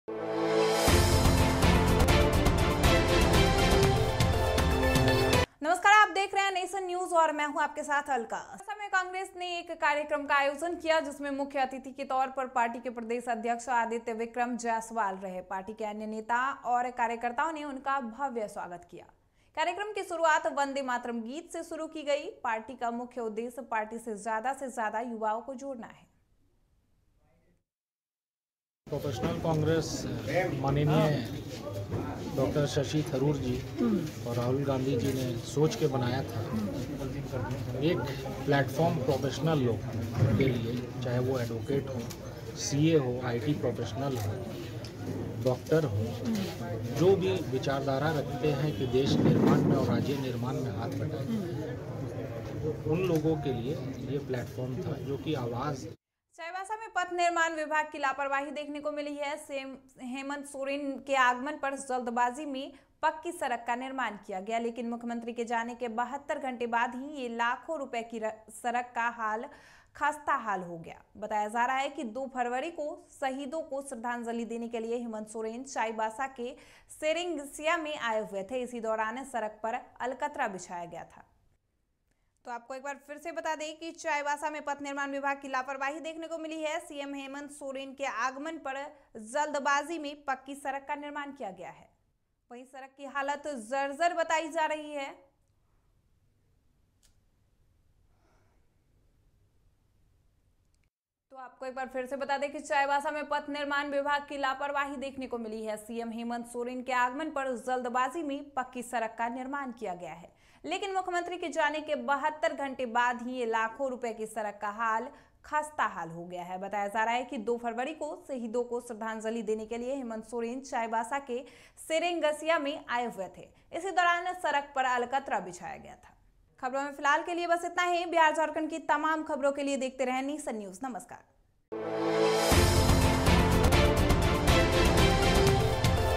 कांग्रेस ने किया कार्यक्रम का आयोजन, सीएम के आगमन पर बना सड़क
चाईबासा में कांग्रेस ने एक कार्यक्रम का आयोजन किया. जिसका मुख्यअतिथि पार्टी के प्रदेश अध्यक्ष आदित्य विक्रम जायसवाल रहे..
सीएम के जाते ही सड़क का हाल हुआ खस्ताहाल

NATIONNEWS NATIONREVIEW JHARKHANDNEWS JHARKHANDLATESTNEWS JHARKHANDTOPNEWS JHARKHANDBREAKINGNEWS CONGRESS KARYAKARM#SADAKNIRMAN